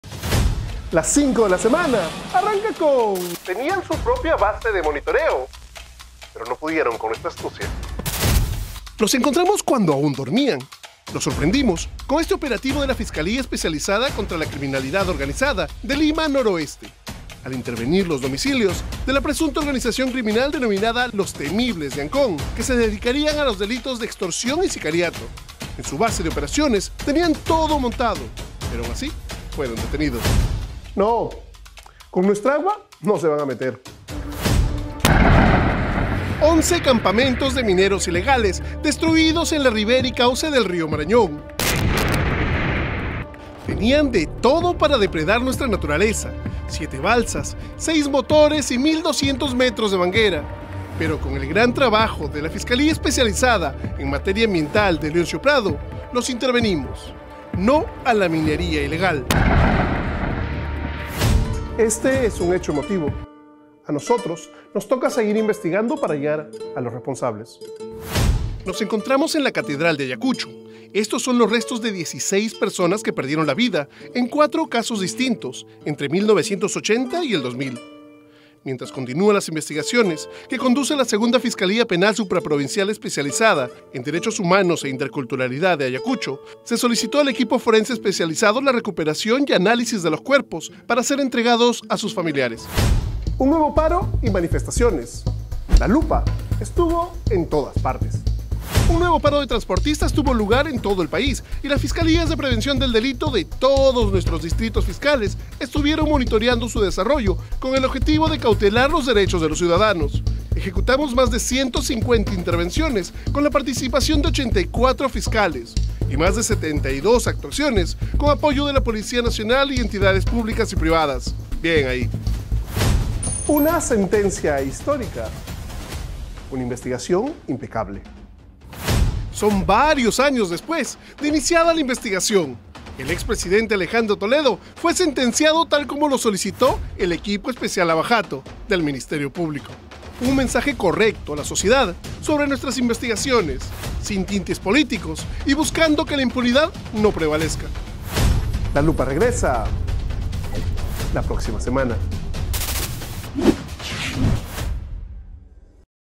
Las5delMP - Edición 36
Las5delMP | ¡Conoce las noticias más importantes de la semana que tenemos para ti desde el Ministerio Público. ¡Ven y acompáñanos en nuestra edición 36! 🙌🏻🎥
Las 5 de la semana.